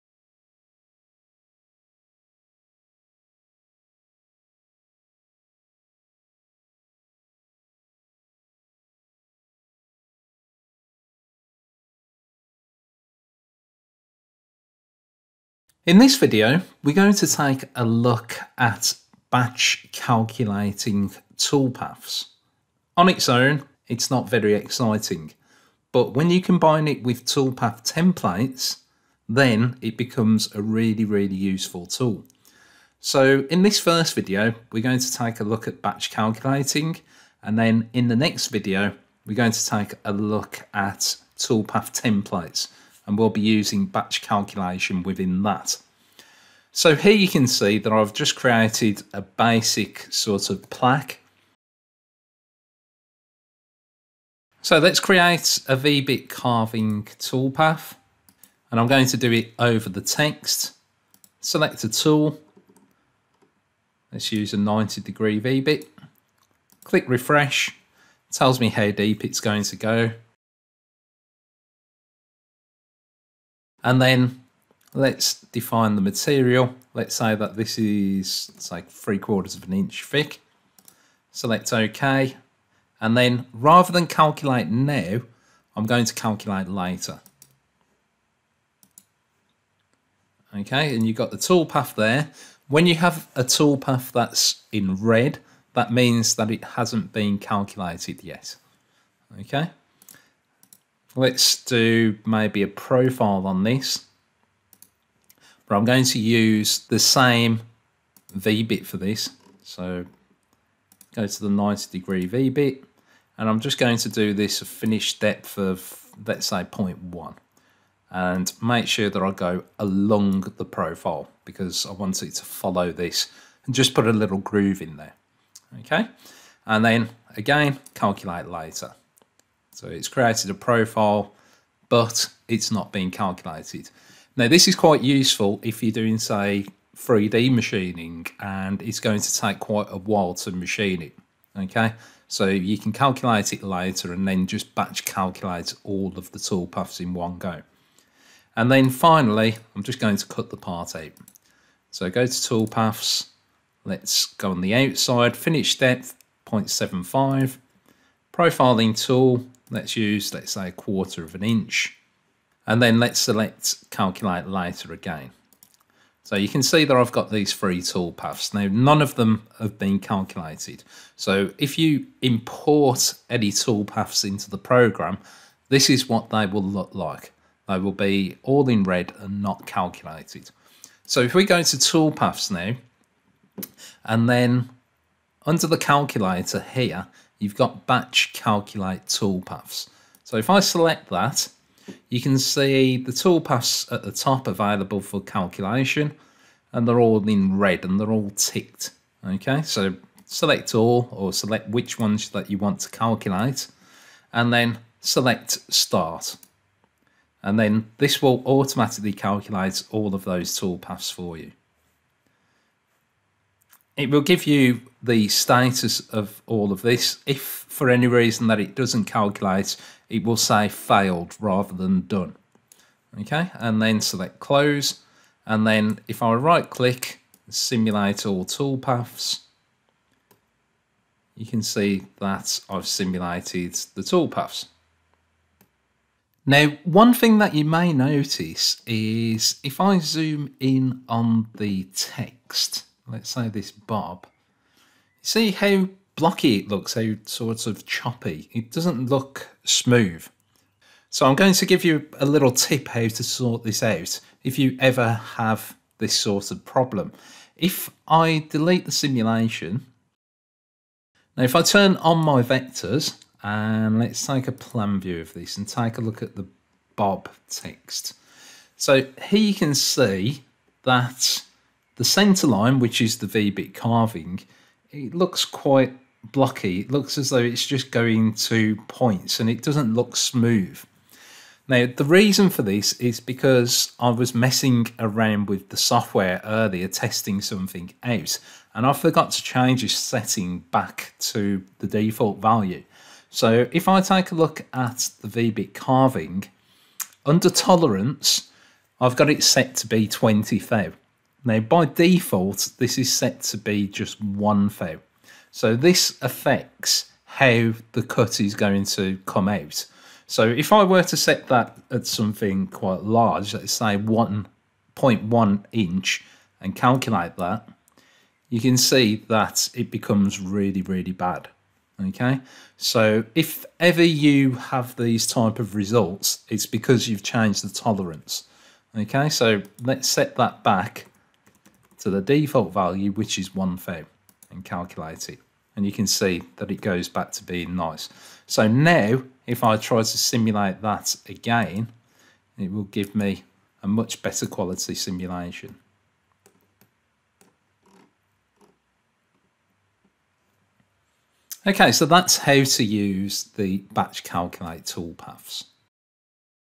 Carveco Maker Plus: Batch Calculate
The Batch Calculate Tool is really useful for working out all of your toolpaths at once, rather than having to manually and edit every toolpath individually. It's perfect for users wishing to save on time and effort and to focus on their awesome designs rather than the nitty-gritty. Leighton walks us through the tool, showing us how to calculate every toolpath with the click of a button.

Bookmarks:
00:00 - Introduction
00:53 - V-Bit Carving Toolpath
01:40 - Profile Toolpath
03:00 - Batch Calculate Toolpaths
03:45 - Simulation
04:03 - Solving Jagged Edges with Tolerance